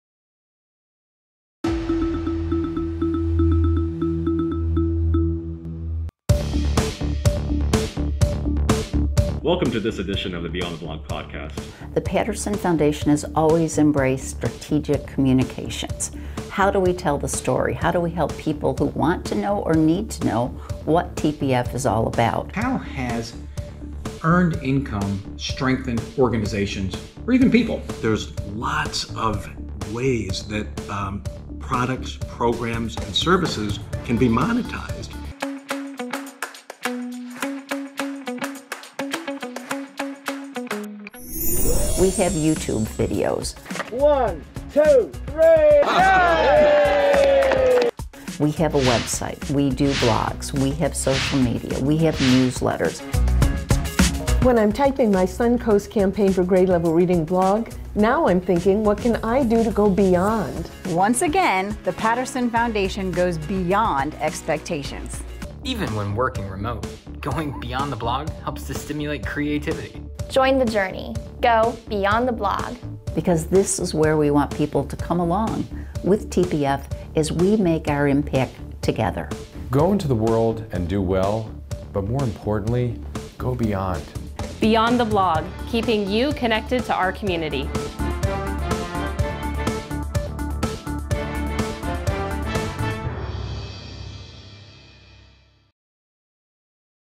TPF Beyond the Blog Promo
Coming Soon… Beyond the Blog, strategic communications evolution of The Patterson Foundation’s storytelling approach focused on creating and sharing engaging content featuring the people, organizations, and communities strengthened through the Foundation’s work.